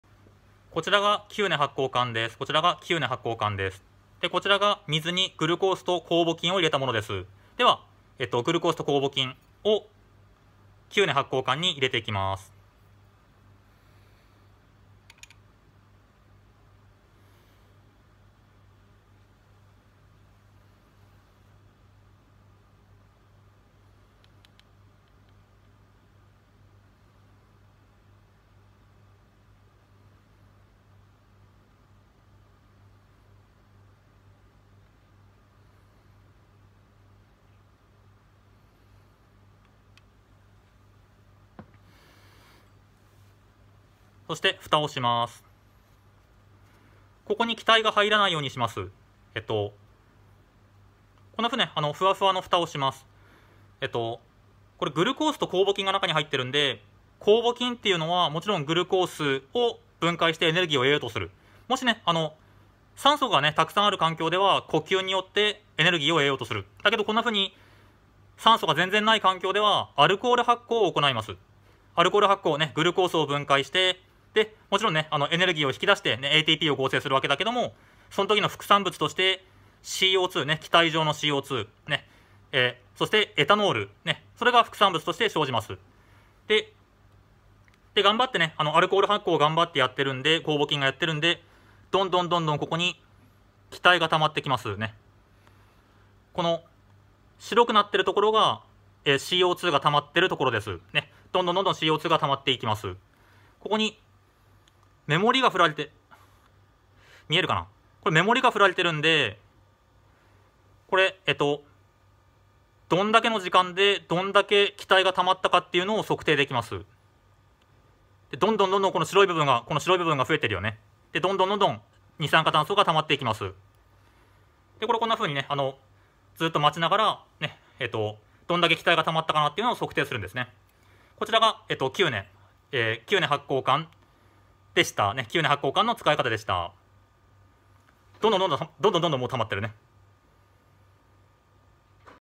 キューネ発酵管【アルコール発酵の観察】 高校生物
キューネ発酵管について解説します（酵母菌を用いてアルコール発酵の観察実験を行います）。実験中ずっとパンのにおいがします（アルコール発酵で生じるエタノールが主なにおいの原因です）。
●キューネはドイツ生理化学の創始者の一人。その業績は計り知れない（神経の両方向伝導の証明、ミオシンの発見、トリプシンの命名、ロドプシンの吸光に伴う変化の発見、酵素の概念の樹立）。酵素一般をエンザイムenzyme（酵母の中にあるものという意味）と命名したのはキューネである。

問題１：アルコール発酵の化学式をかけ。
答え：C6H12O6→2CO2+2C2H5OH

問題２：問題１の反応では、グルコース１分子あたり何分子のATPが生成するか。
答え：２分子

問題３：動画のような実験を行った後、水酸化ナトリウムを加えよく混ぜると、キューネ発酵管内の圧力が減少した（指を開口部に当てておくと、指が管内に吸いつけられた）。これは、水酸化ナトリウムが（　　　）を吸収したためである。空欄を埋めよ。
答え：二酸化炭素

問題４：動画のような実験を行った後、盲管部から溶液を取り出して、ヨウ素ヨウ化カリウム溶液を加え、６０～７０度に加熱した。すると、液が黄変し、ヨードホルムのにおい（消毒薬臭）がした。このことから、アルコール発酵によってある物質ができたことがわかった。ある物質とは何か。
答え：エタノール
＊ヨードホルム反応でエタノールがあるかどうか調べている。

問題５：酵母菌はミトコンドリアを持つか？持たないか？
答え：持つ（酵母菌は真核生物である。真核生物はミトコンドリアを持つ）。

問題６：酸素が存在すると、発酵が抑制される現象を何というか。
答え：パスツール効果（発酵速度が好気的条件下で抑制される現象をパスツール効果という。酸素が存在し、酸素を使って呼吸を行うことができるならば、１分子のグルコースから呼吸では最大３８ATPが生成されるので、アルコール発酵より呼吸を行った方が効率的である）

＊ガス捕集部分についた目盛で生成するガス量を『簡易的に』知ることができますが、この器具は、発酵強度を『精確に』測定するのには適していません。
＊綿栓で蓋をせず、きつく密閉してしまうと、内部で気体（CO2）が発生しているので、破裂する恐れがあり、危険です。
＊今回は、概念だけを示したいので、少し適当に実験しています。
実際の実験方法（例）
① １０[g]のパン酵母を１０[％]グルコース水溶液１００[mL]に入れる。
② よく混ぜ、１０分程度放置する。
③ 気体が発生してきたら、キューネ発酵管の盲管部（上に伸びている閉じているところ）に隙間なく溶液を注ぐ。
④ 盲管部の上部にたまる二酸化炭素の体積を１分間隔で測定する。

●アルコール発酵に酵母菌が関係することは、シュワン（動物についての細胞説を提唱したことで有名な動物生理学者。胃液からペプシンを発見者したのも彼である）が見抜いていた。その後、パスツール（自然発生説の否定で有名な微生物学者）が、糖などを含む培地で酵母菌を培養すると、糖が消費されてアルコールができることを確認した（ちなみに、発酵が酸素によって抑制される現象を『パスツール効果』と言う）。